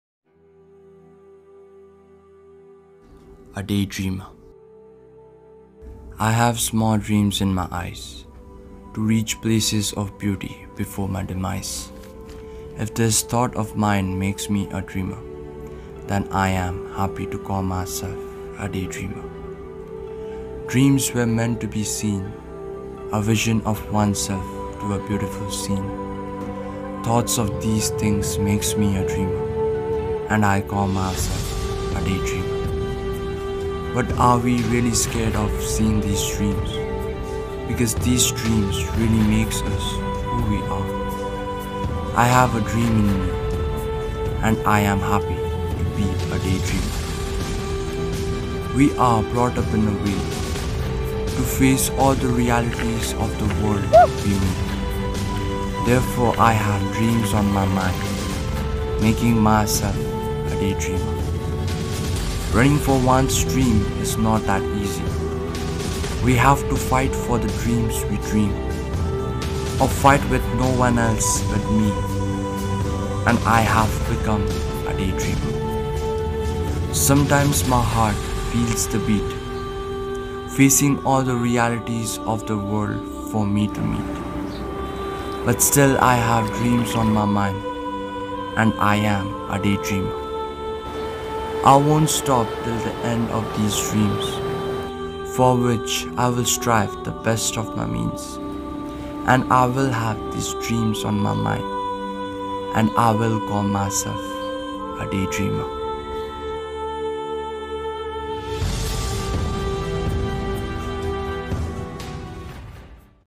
Cinematic view | On a cycle | Gajaldoba teesta barrage | picnic spot | Dooars
Cinematic view of my solo cycling trip to Gajoldoba which was around 70km.

It was my first time visiting that  place and I really  enjoyed everything I saw during my ride.

Hope I can justify the views I saw with my cinematography.

Also a small narration of my poem " A Day Dreamer ".

Until then see you guys.